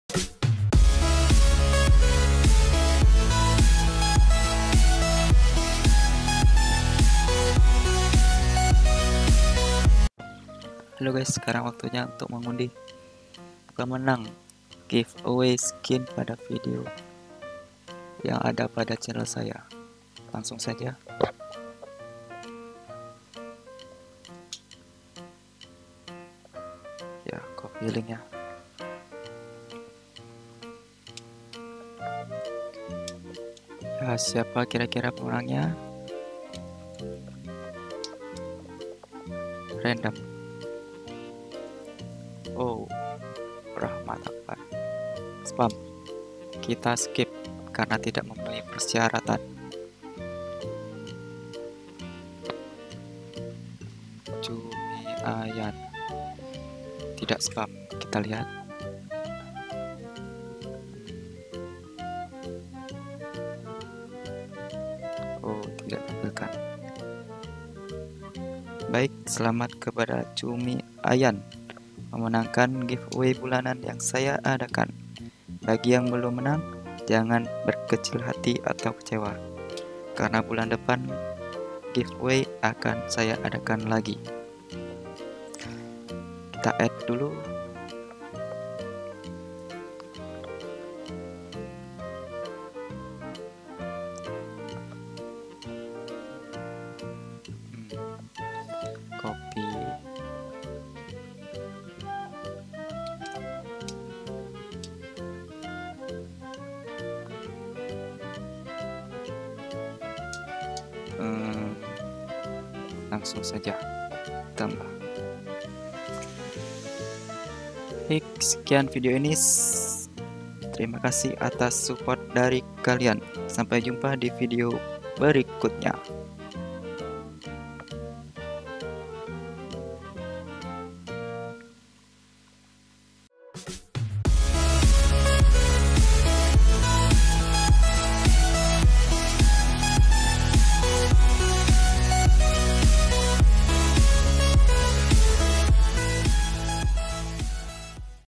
Pengundian Give Away Skin 269 DIA | Mobile Legends Indonesia #1
Halo kawan,
Ini dia pemenang Give away skin yang saya adakan di bulan Februari.
Bagi yang lain jangan kecewa karena bulan depan Give away akan saya adakan lagi kok :)
Tetap Support saya ya
Thanks